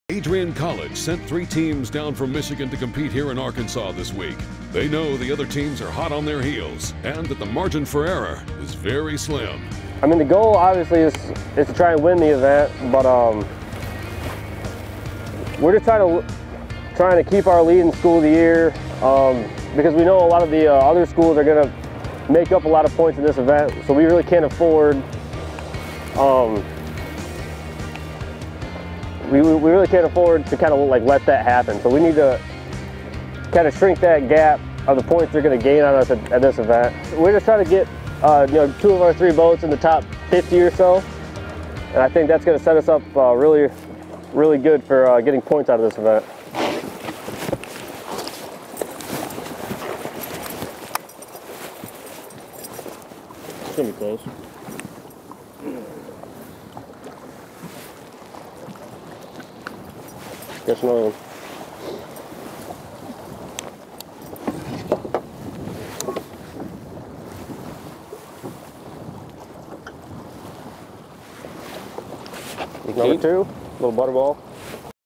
Adrian College at 2019 AFTCO Collegiate Bass Open
Adrian College entered the 2019 AFTCO Collegiate Bass Open in 1st place for the Bass Pro Shops School of the Year presented by Abu Garcia.  Our camera crew caught up with a team of Adrian College anglers on Day 1.